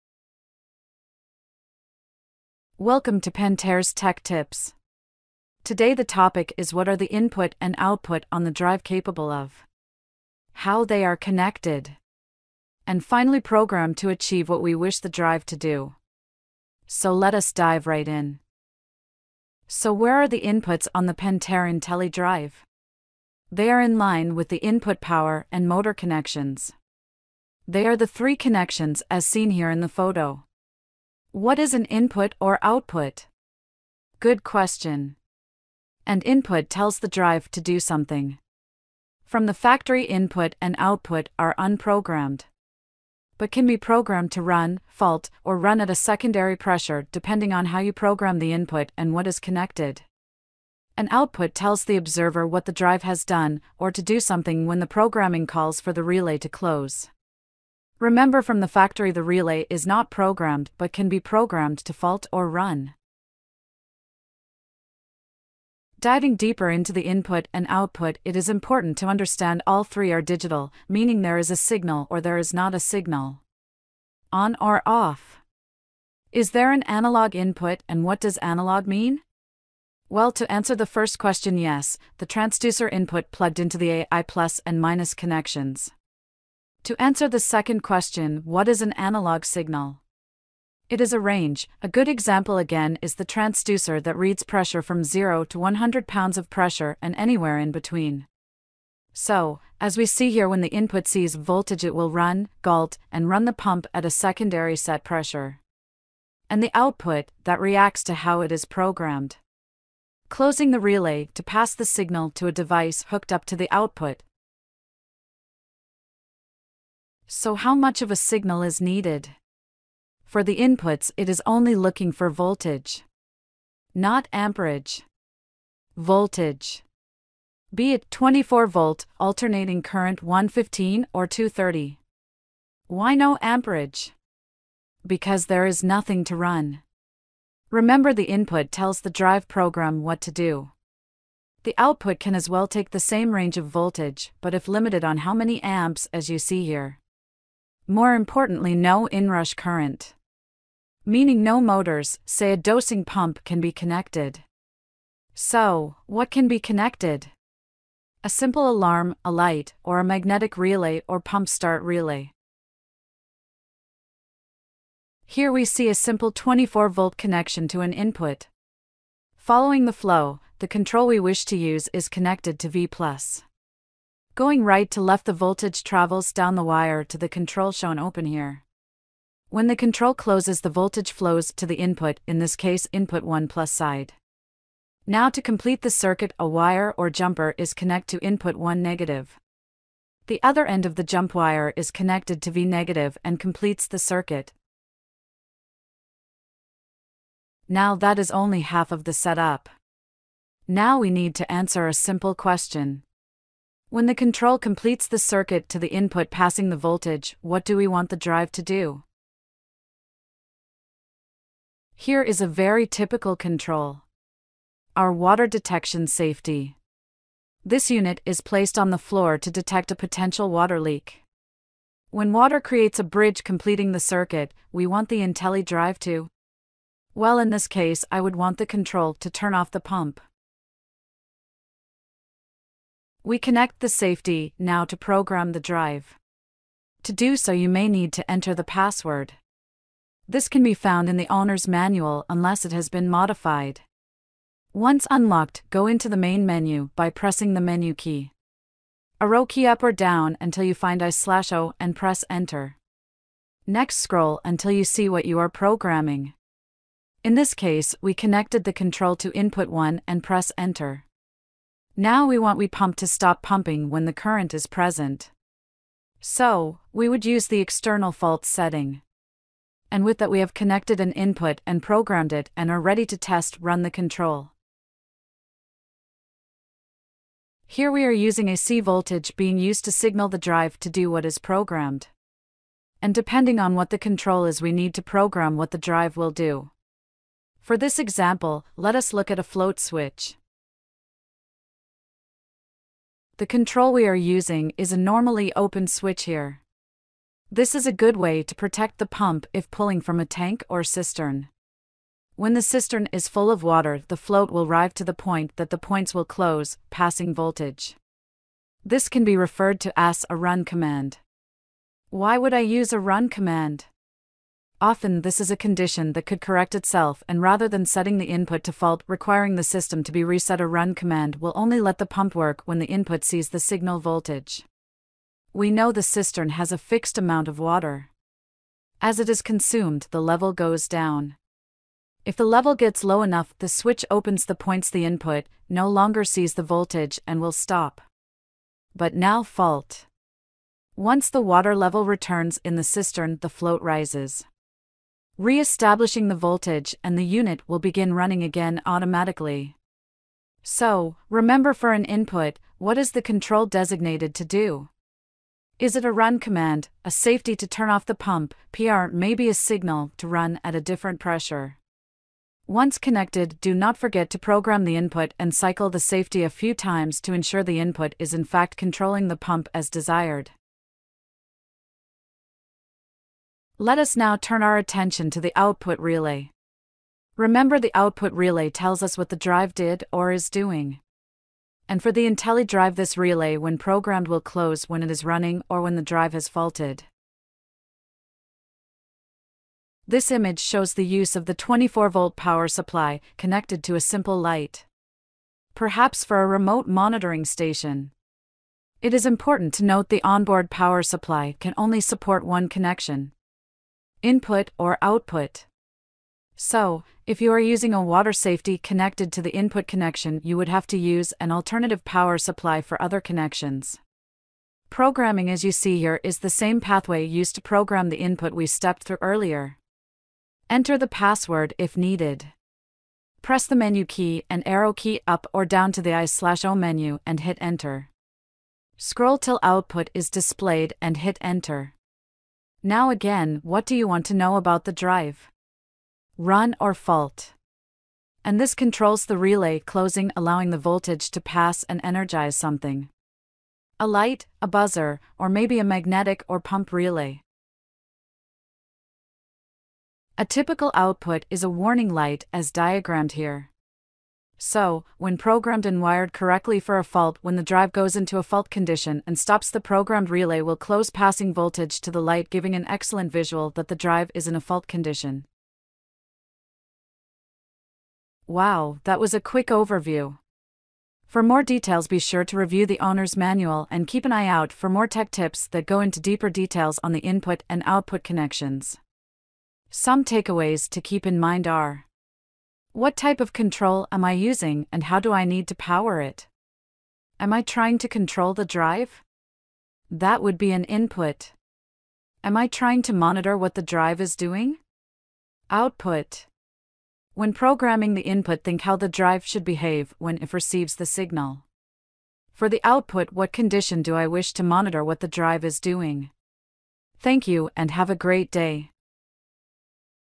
Pentair Tech Tips: Intellidrive Input - Output Review
Pentair Tech Tips presents Inputs and Outputs on the Intellidrive. This episode covers the capabilities of the inputs and outputs, how they are connected, and programming the drive.